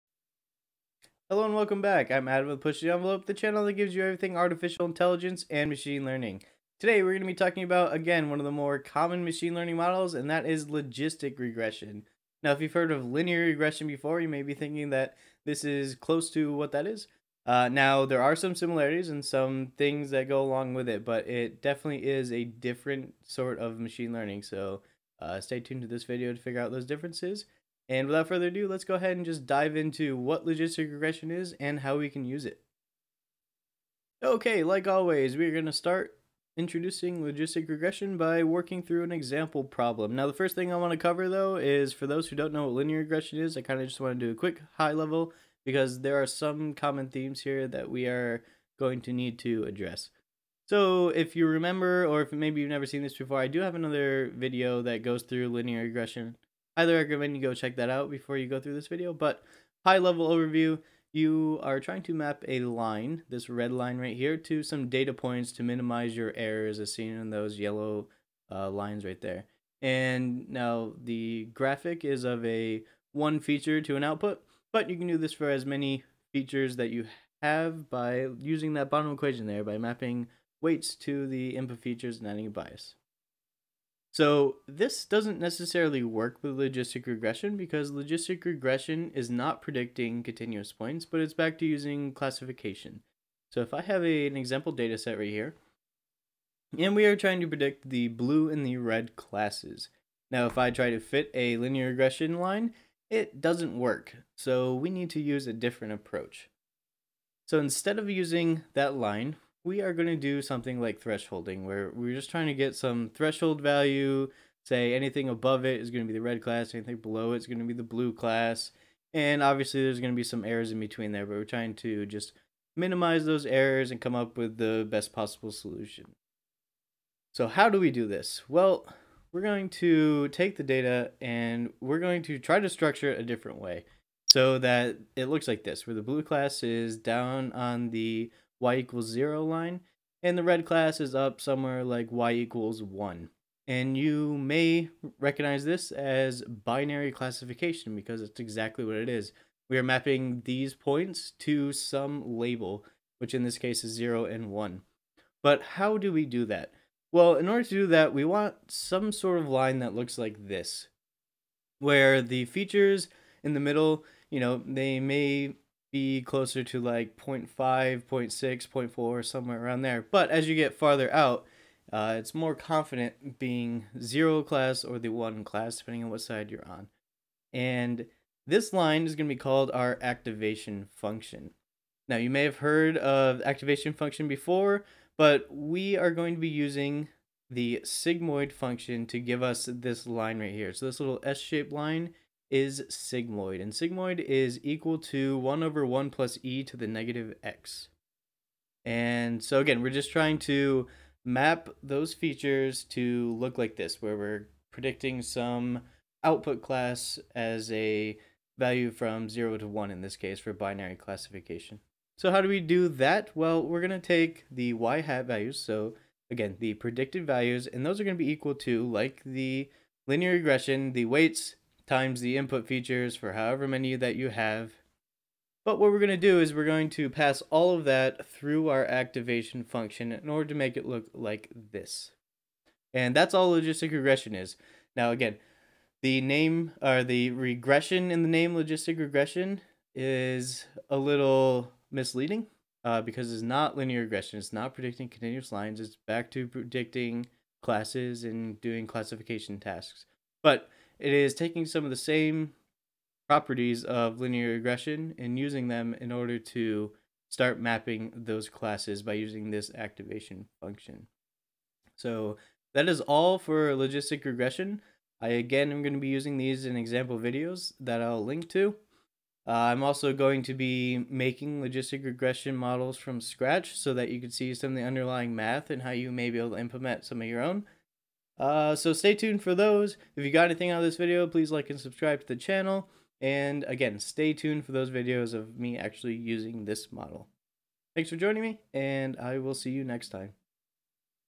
Logistic Regression for Beginners | Machine Learning in Plain English
Introduction to Logistic Regression in Machine Learning for beginners. Video will focus on how Logistic Regression works and when it can be used for making predictions.